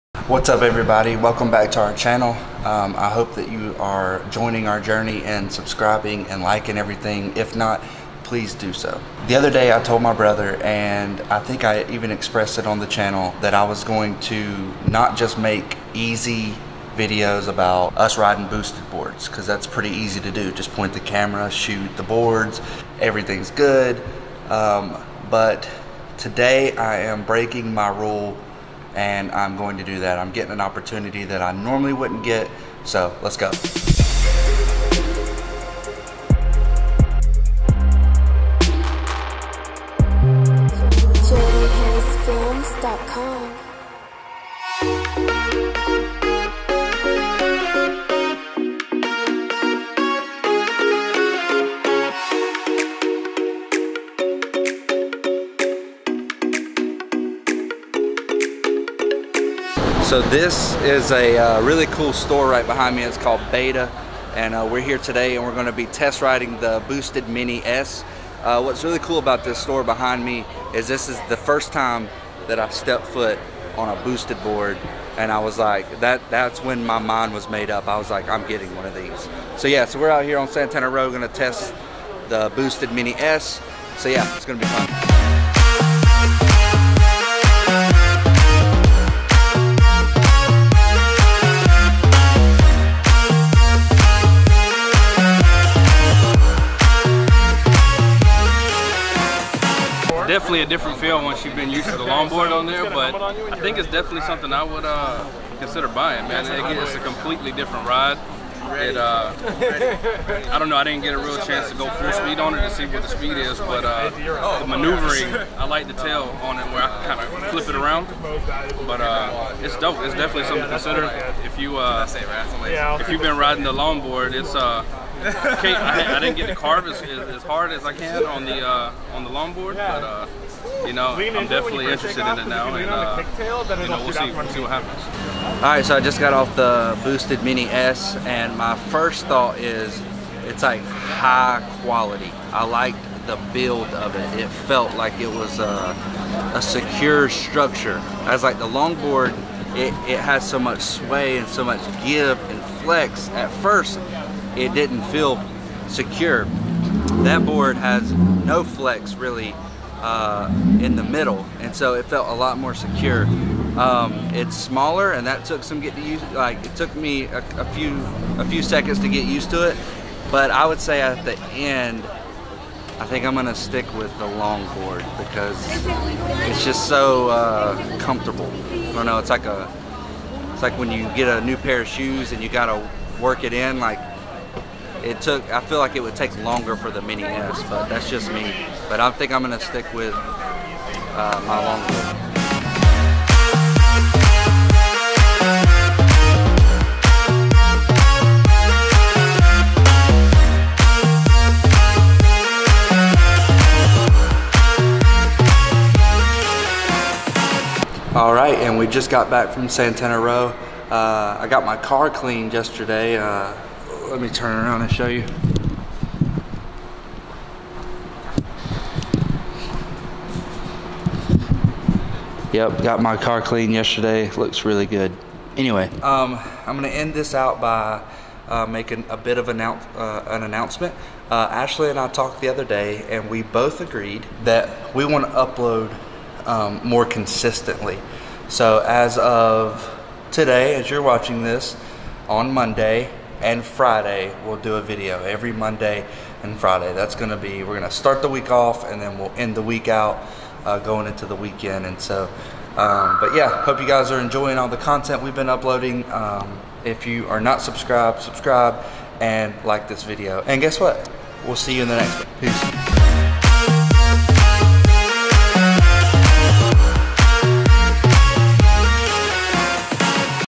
Boosted Mini S Review and BIG NEWS
Today AJ and I got to test the Boosted Mini S!